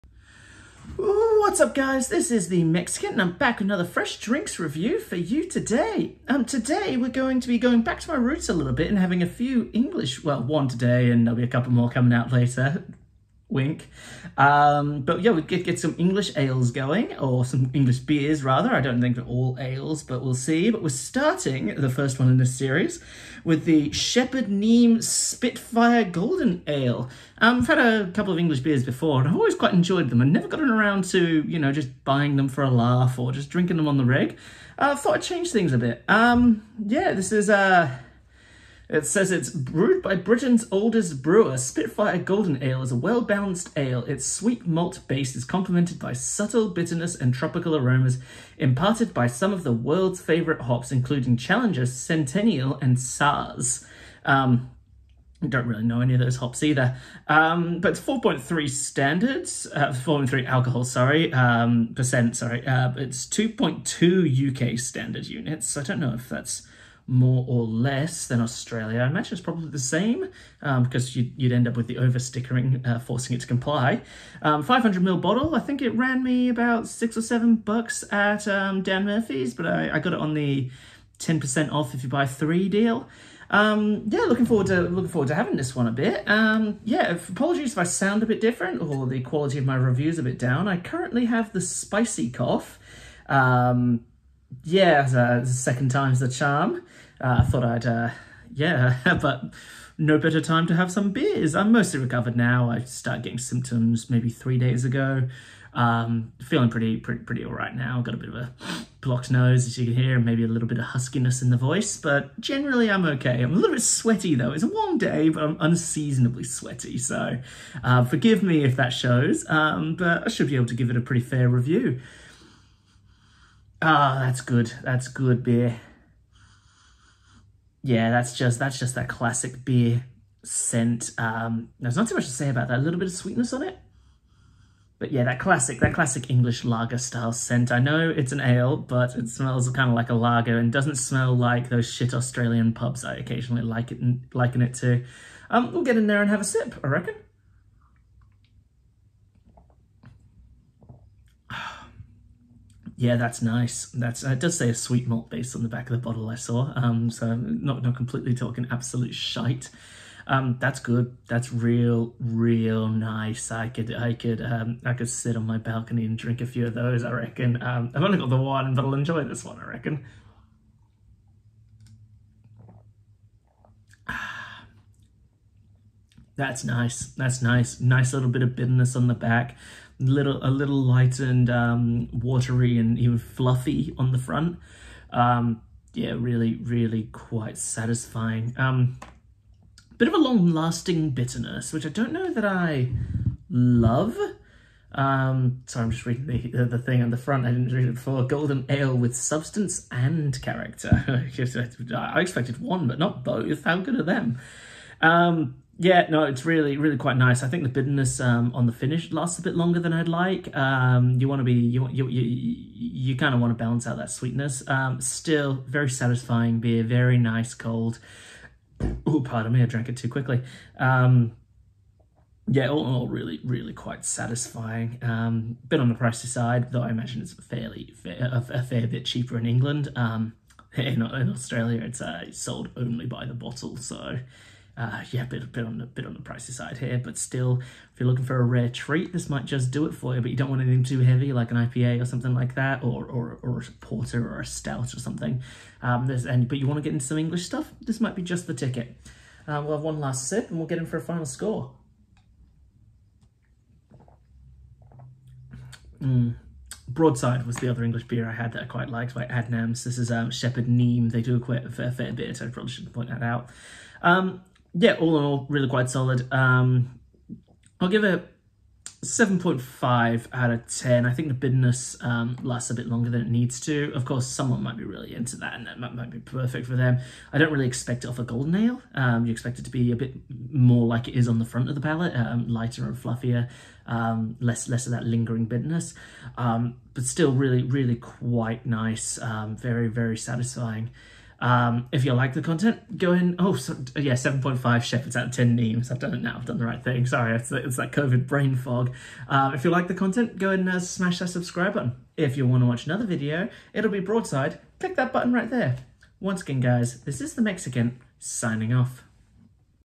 Spitfire golden ale
Welcome to the first in a 3 part series on some British beers I picked up. Lets see how they fare